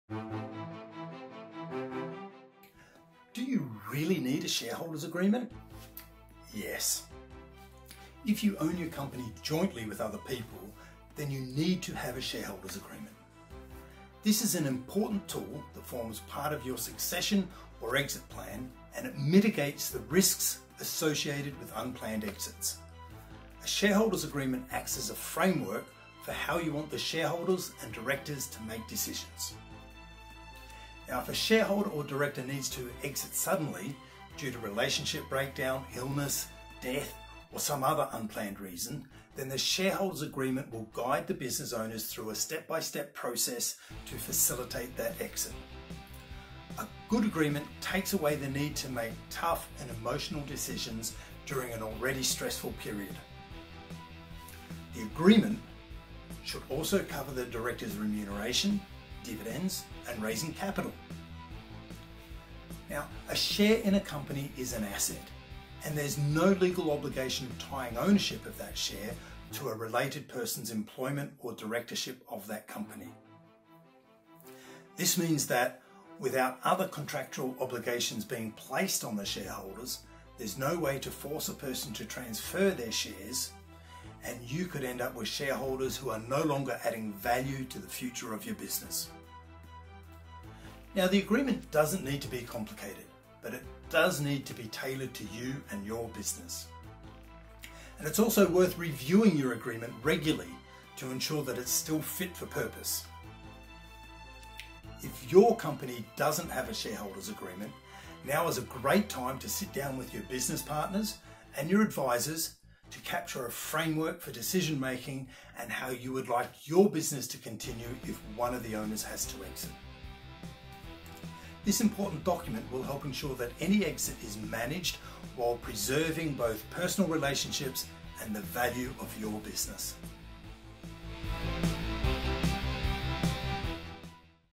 Do you really need a shareholders agreement
Do you really need a shareholder's agreement? Yes.

If you own your company jointly with other people, then you need to have a shareholders' agreement. This is an important tool that forms part of your succession or exit plan, and it mitigates the risks associated with unplanned exits.

A shareholders' agreement acts as a framework for how you want the shareholders and directors to make decisions. Now, if a shareholder or director needs to exit suddenly due to relationship breakdown, illness, death, or some other unplanned reason, then the shareholders' agreement will guide the business owners through a step-by-step process to facilitate that exit.

A good agreement takes away the need to make tough and emotional decisions during an already stressful period. The agreement should also cover the directors' remuneration, dividends, and raising capital. Now, a share in a company is an asset, and there's no legal obligation of tying ownership of that share to a related person's employment or directorship of that company. This means that without other contractual obligations being placed on the shareholders, there's no way to force a person to transfer their shares, and you could end up with shareholders who are no longer adding value to the future of your business.

Now, the agreement doesn't need to be complicated, but it does need to be tailored to you and your business, and it's also worth reviewing your agreement regularly to ensure that it's still fit for purpose. If your company doesn't have a shareholders' agreement, now is a great time to sit down with your business partners and your advisers to capture a framework for decision-making and how you would like your business to continue, if one of the owners has to exit. This important document will help ensure that any exit is managed while preserving both personal relationships and the value of your business.